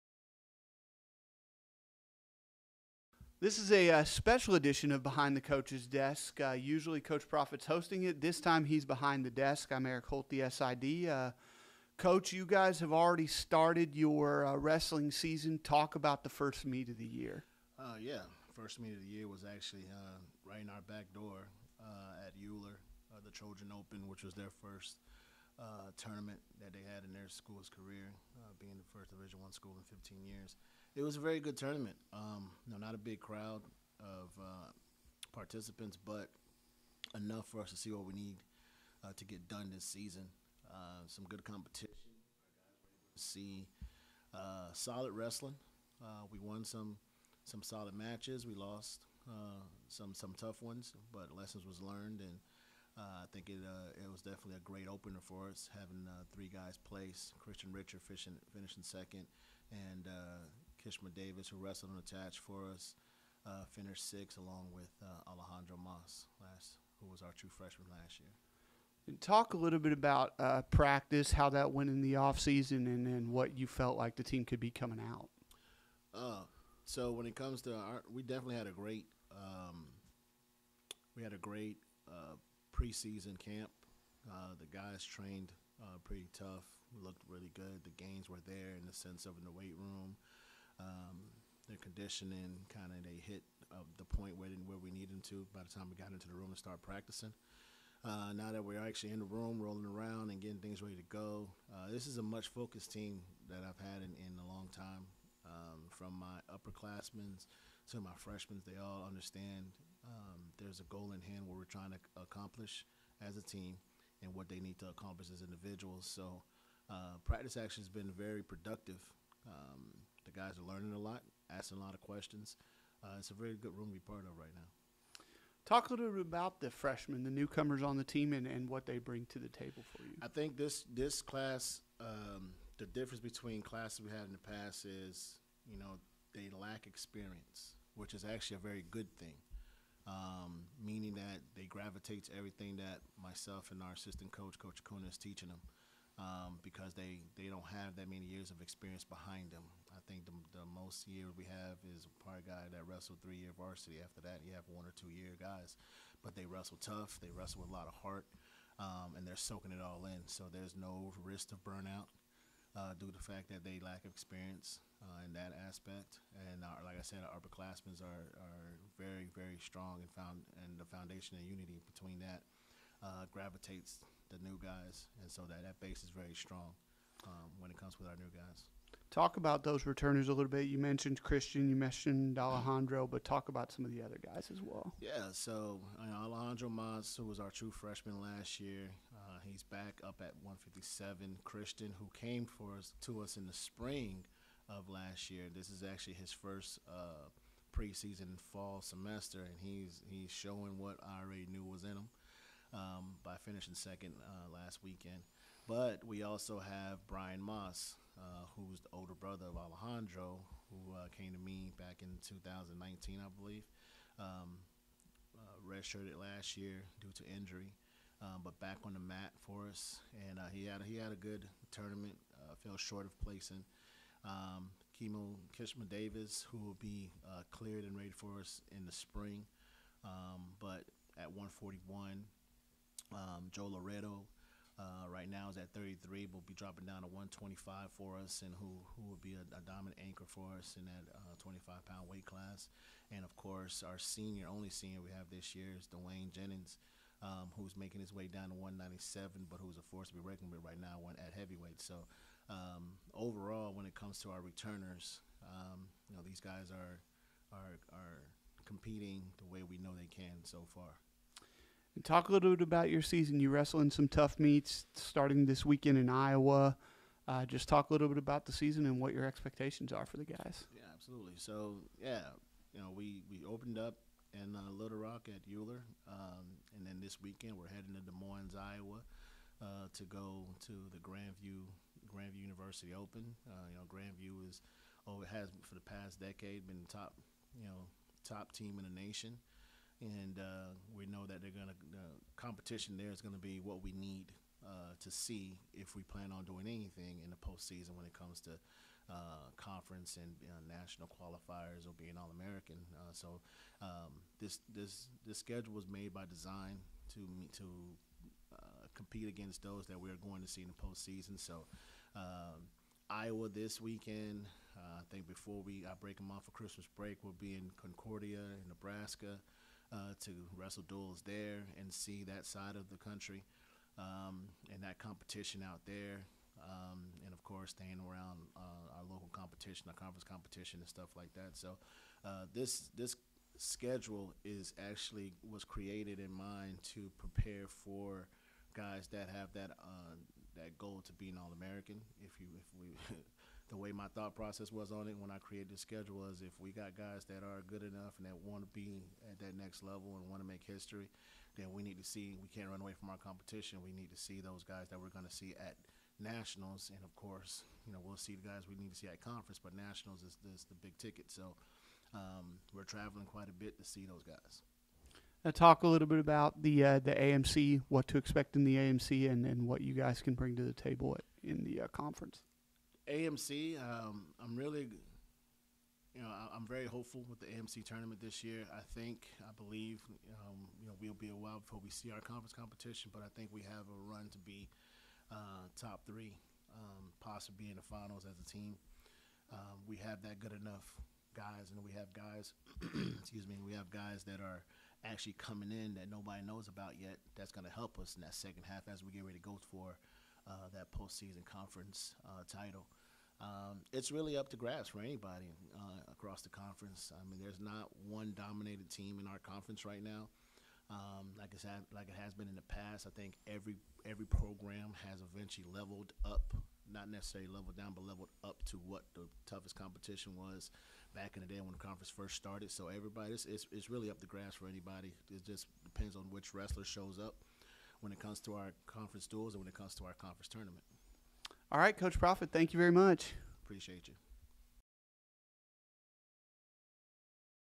Behind the Desk w Ken Prophete - Wrestling Season Preview 2021-22
In this special edition of Behind the Desk, Sports Information Director Erik Holth sits down with Ken Prophete to talk about the upcoming season for the wrestling program. For more information on CBC Athletics, visit cbcmustangs.com, Facebook.com/gocbcmustangs and follow @gocbcmustangs on Twitter and Instagram.